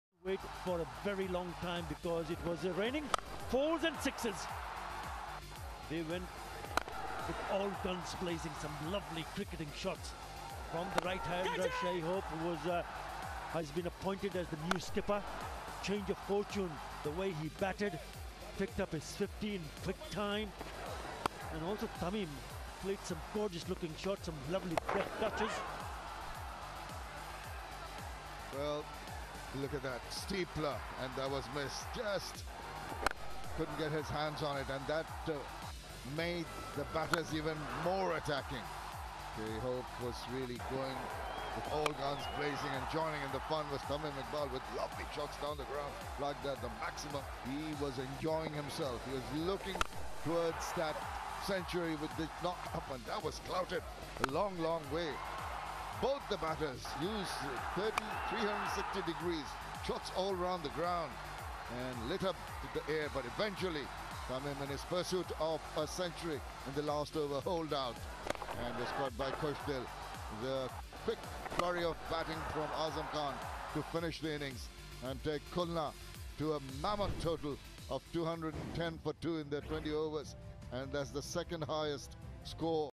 Comilla Victorians vs Khulna Tigers highlights | BPL2023 match 32 Highlights
Comilla Victorians  vs Khula Tigers highlights | BPL2023 match 32 Highlights | khulna tigers vs Comilla victorians 1st inning full short highlghts. Bangladesh premier league 2023 today Match 32nd highlights of Khulna tigers batting. Today match highlights of 1st inning.